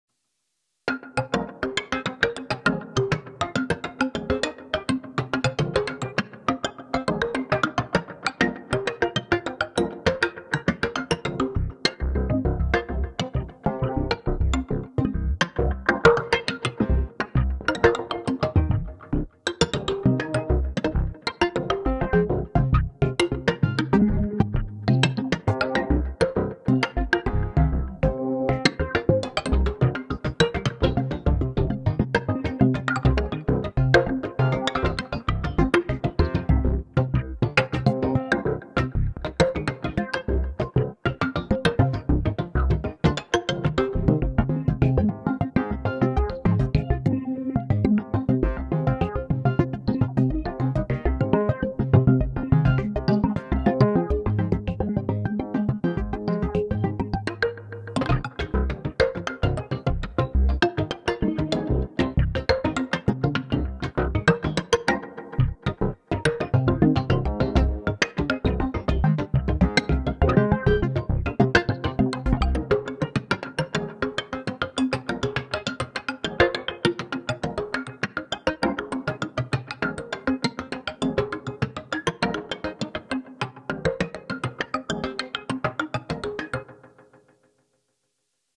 Drum Solo on the Sensory Percussion "Lasko"
Sensory Percussion preset "Lasko" kit from the "Black Cat" sound pack.